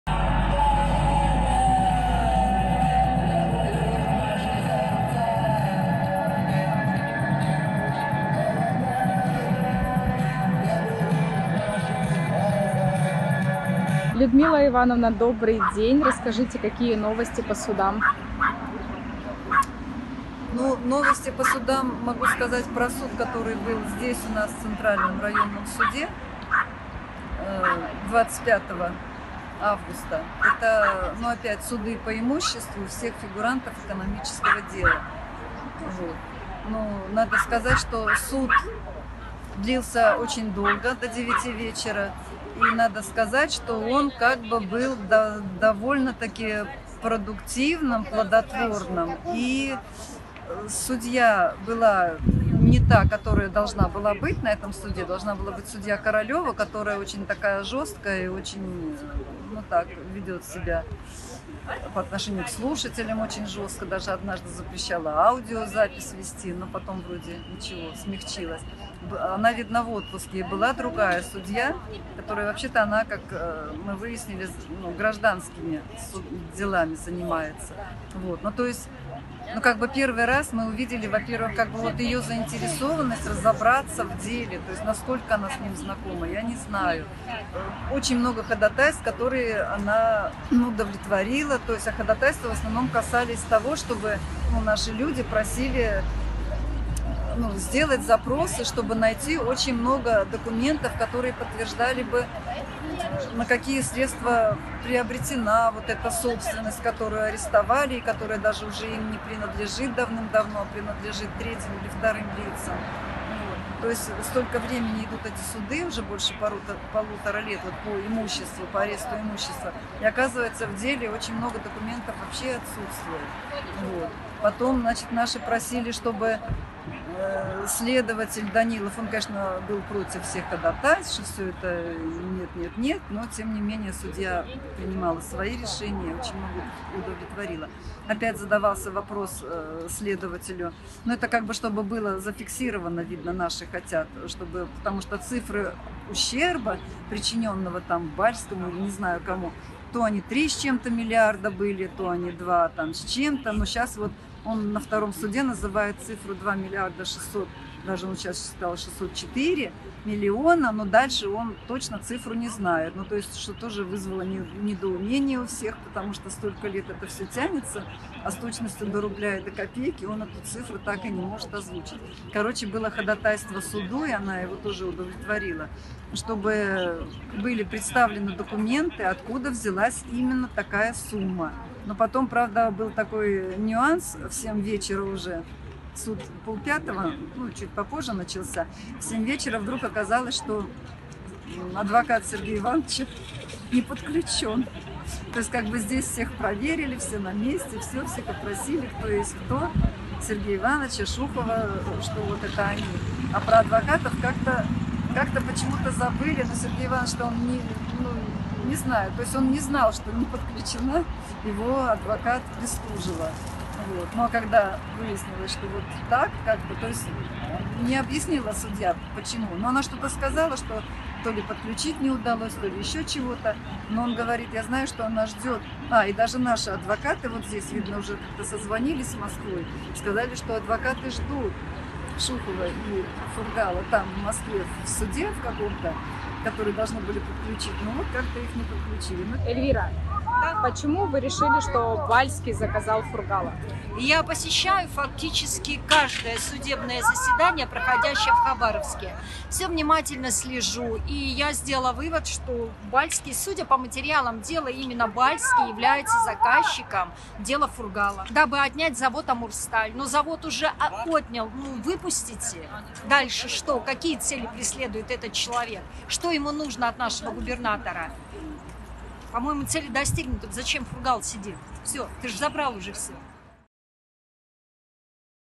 🟠447-й день Бессрочного пикета в Хабаровске
"В семь вечера вдруг оказалось, что адвокат Фургала не подключён к заседанию суда"

447-й день Бессрочного пикета в Хабаровске в защиту Сергея Фургала и всех политзаключённых.